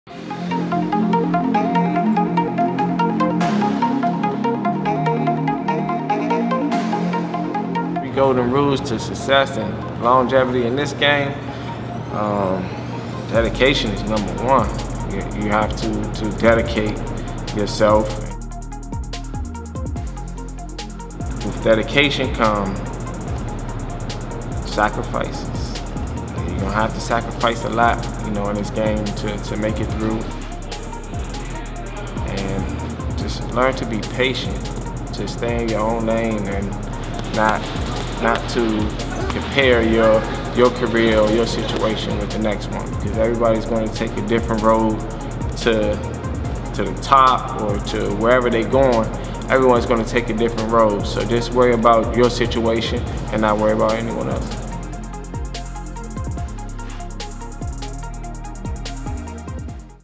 Lamont Peterson Talks Longevity & Success
After a lengthy 16-month hiatus, former 140-pound world champion, Lamont Peterson sits down with PBC to share his story on how he has managed to maintain a successful 13+ year career. Here are this three golden rules!